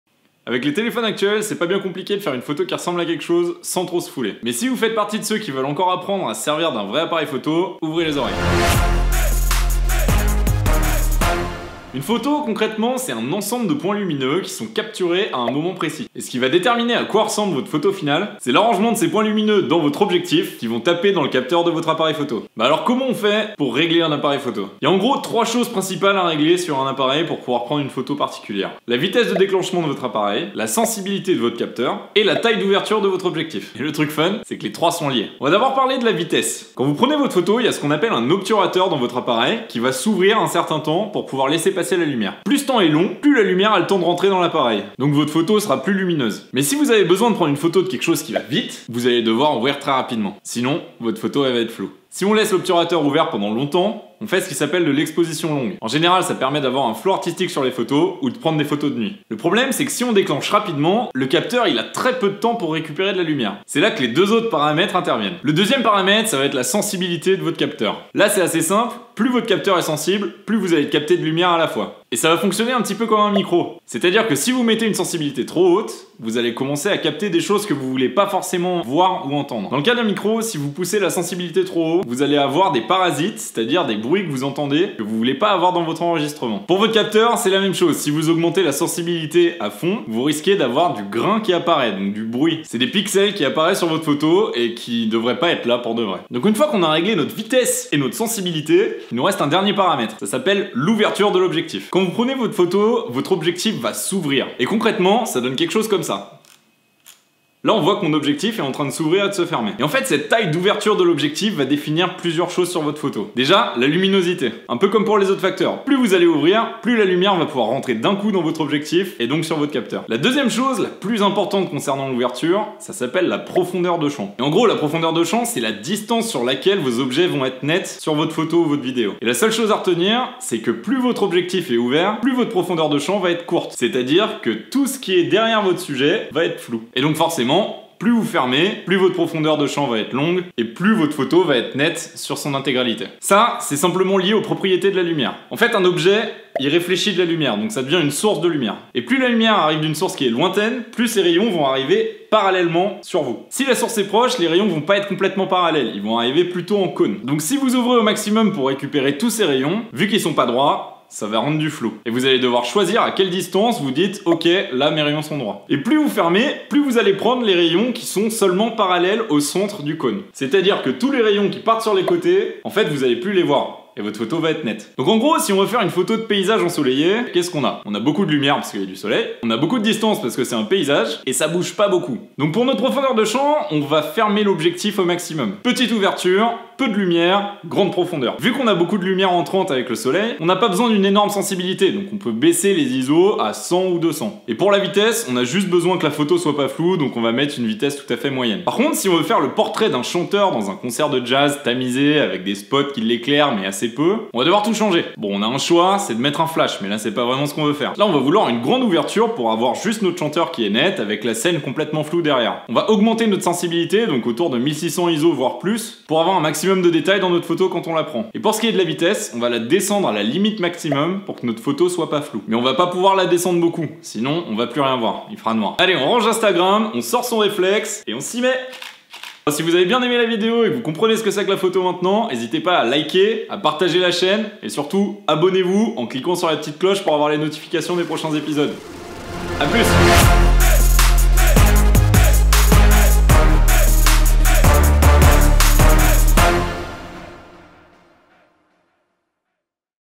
LES BASES DE LA PHOTOGRAPHIE: Comment régler son appareil photo ?
Vous avez toujours voulu vous mettre à la photo mais jamais osé ? Vous êtes un pro d'instagram et voulez passer à la vitesse supérieure ?

On explore les bases de la photo en quelques minutes : vitesse, ouverture et sensibilité, et comment on fait pour régler son appareil photo.